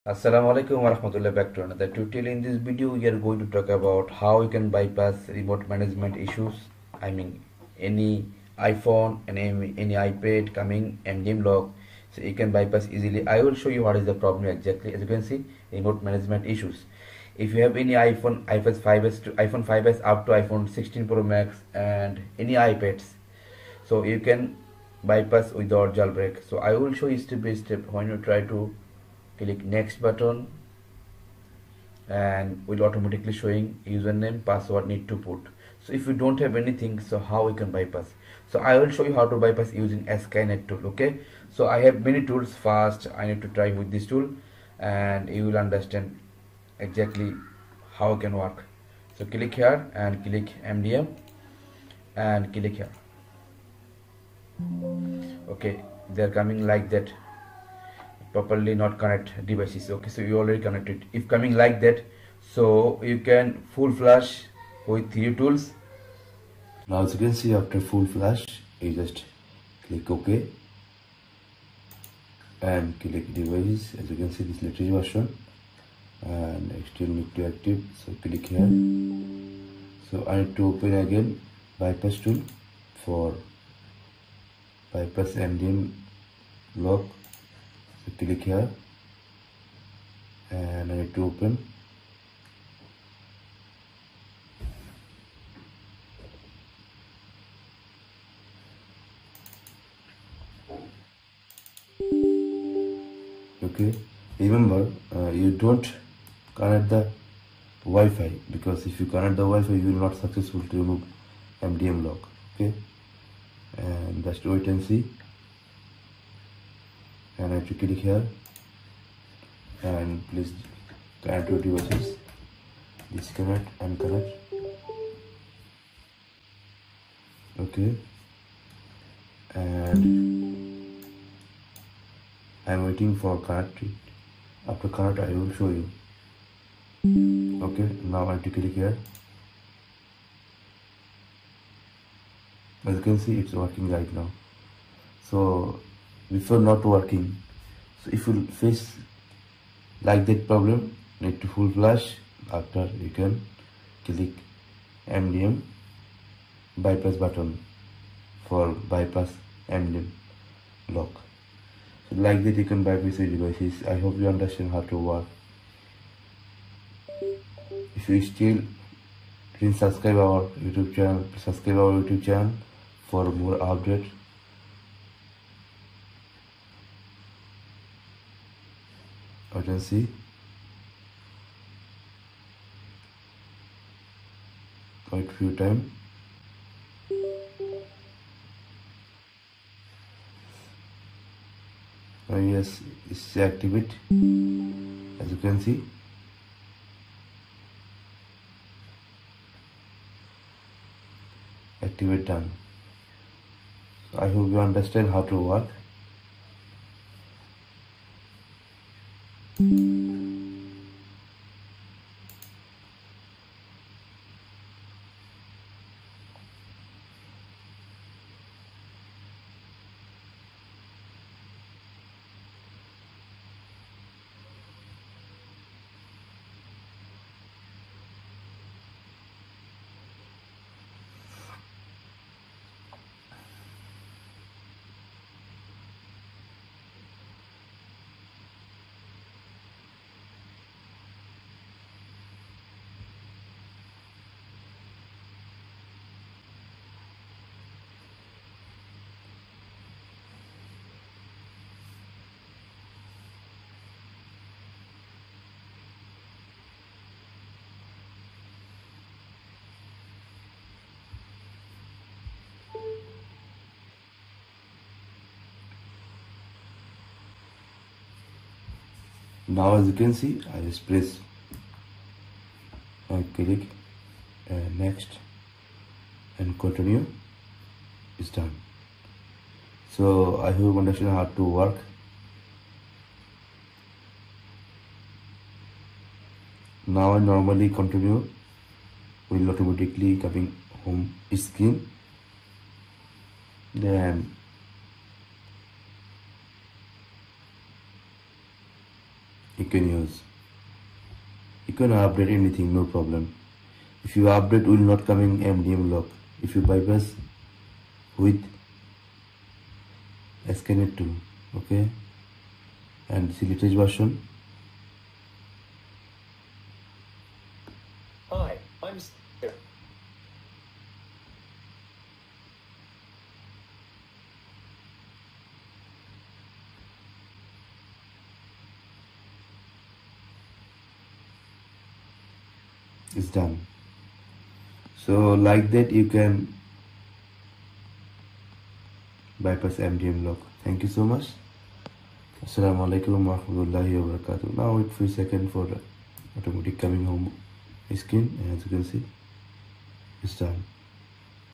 All iPhone and iPad remote management Bypass iPhone 16 Pro Max MDM Bypass
if you have any iPhone or iPads Remote Management Problem any iOS version you can bypass with skynettool without jailbreak just one click after active best price in bestofunlock.com